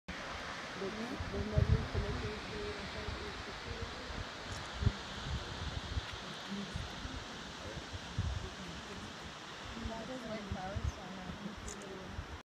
Thousand Foot Falls, Mountain Pine Ridge, Belize
A panning clip of Thousand Foot Falls national park in the Cayo district of Belize.  This waterfall has a 1000' drop, a large pool, followed by another 600' waterfall!